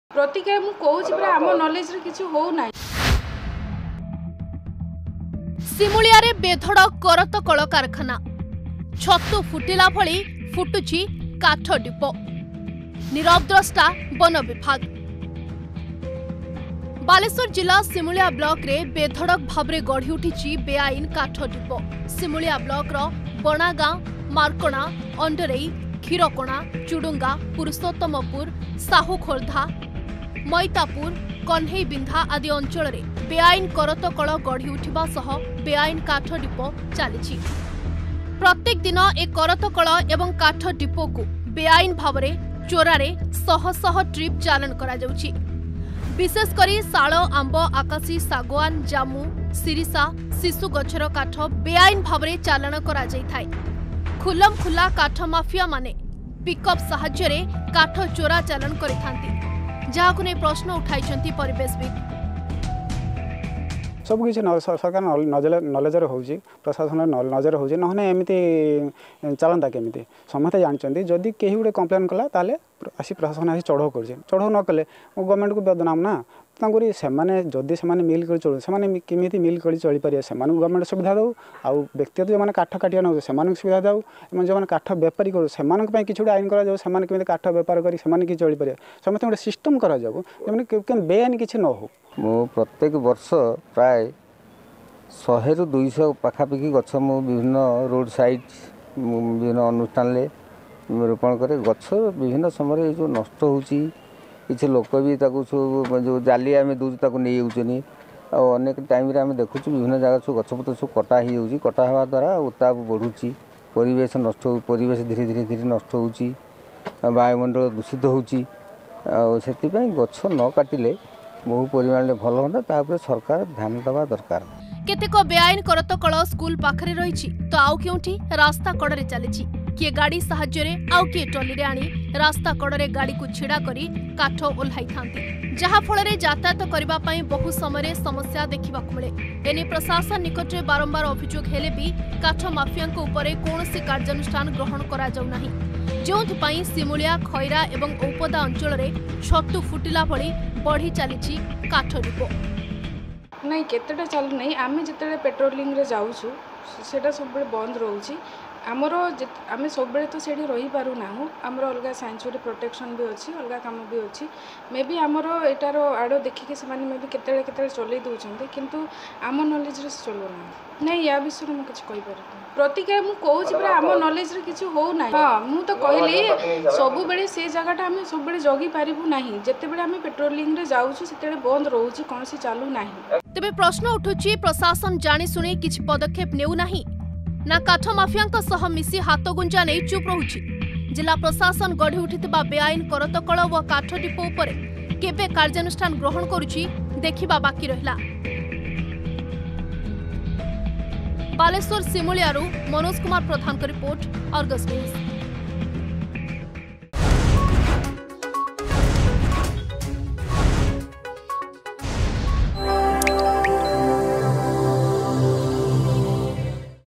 ସିମୁଳିଆରେ ବେଧଡ଼କ କରତ କଳ କାରଖାନା
ଛତୁ ଫୁଟିଲା ଭଳି ଫୁଟୁଛି କାଠ ଡିପୋ । ବେଆଇନ ଚାଲିଛି ଚୋରାଚାଲାଣ । ନୀରବଦ୍ରଷ୍ଟା ବନ ବିଭାଗ ।ବାଲେଶ୍ୱର ଜିଲ୍ଲା ସିମୁଳିଆ ବ୍ଲକରେ ବେଧଡ଼କ ଭାବେ ଗଢି ଉଠିଛି ବେଆଇନ କାଠ ଡିପୋ.